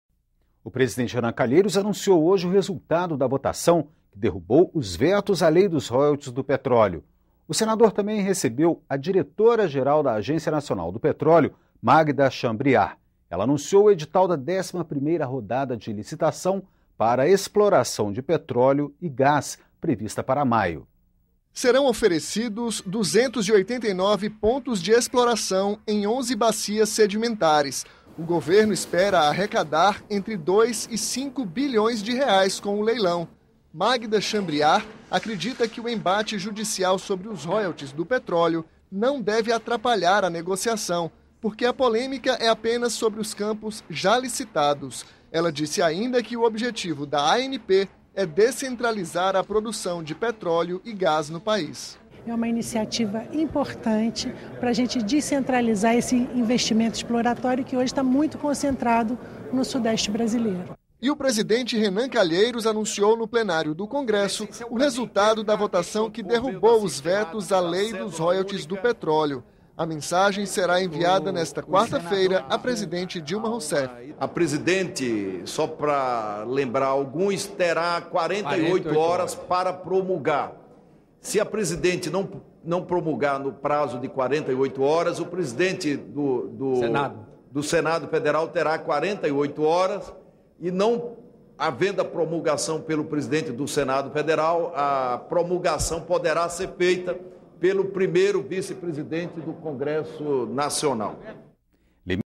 Jornalismo - Resultado da votação que derruba os vetos à lei dos royalties do petróleo - Bloco 1
O presidente do Senado, Renan Calheiros, anuncia o resultado da votação que derruba os vetos à lei dos royalties do petróleo. O senador também recebe nesta terça-feira a diretora-geral da Agência Nacional do Petróleo, Magda Chambriard